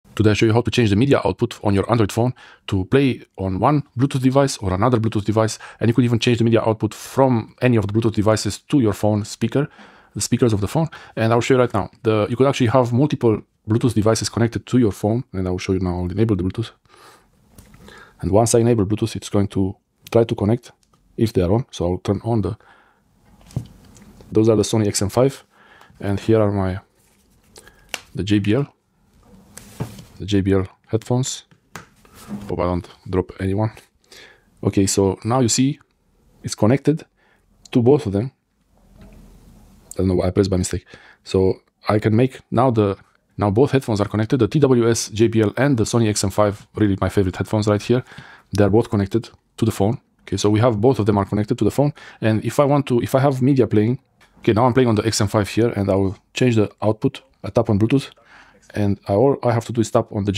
How to Change Media Output on Multiple Bluetooth Devices for Android Phones and Tablets 3
How to Change Media Output on Multiple Bluetooth Devices for Android Phones and Tablets.

How to connect multiple Bluetooth speakers and how to switch between Bluetooth headphones.
Learn how to change media output on Android devices.

In this video, I show you how to change media output on multiple Bluetooth devices using your Android phone. If you want to connect to multiple Bluetooth speakers or headphones and switch between them easily, this step-by-step video guide will help you.

I’ll show you the steps to connect multiple Bluetooth devices to your Android phone and how to switch the media output between them.
Learn how to manage Bluetooth connections, set one device as active for media playback, and easily switch to another device when needed.

Any questions?